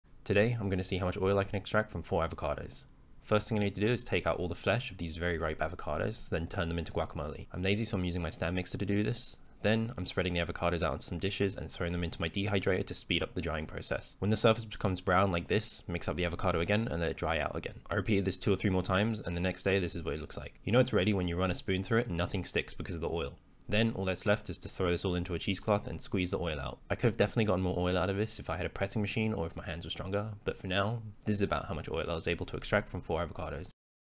How Much Avocado Oil Can I Make From 4 Avocados?
Video Equipment:
Fujifilm X-S10
FUJINON XC 15-45MM F/3.5-5.6
Audio Technica AT2020 w/ Behringer U-Phoria UM
Movo WMX-1 Wireless Lavelier Mic